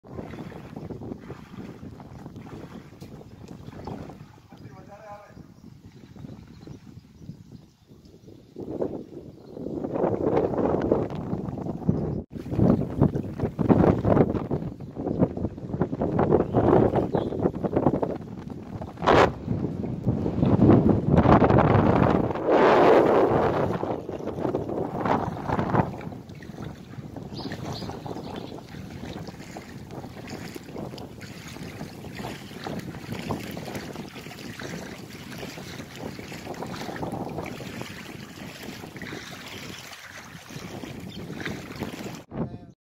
દ્વારકા પોરબંદર રોડ પર આવેલ હરસિઘ્ઘી માતાજી ના મંદિર પાસે વરસાદી પાણી થી દુકાનો માં પાણી આવી ગયા હતા
દ્વારકા પોરબંદર રોડ પર આવેલ હરસિઘ્ઘી માતાજી ના મંદિર પાસે વરસાદી પાણી થી દુકાનો માં પાણી આવી ગયા હતા મંદિર પાસે પાણી પાણી થઈ ગયું હતું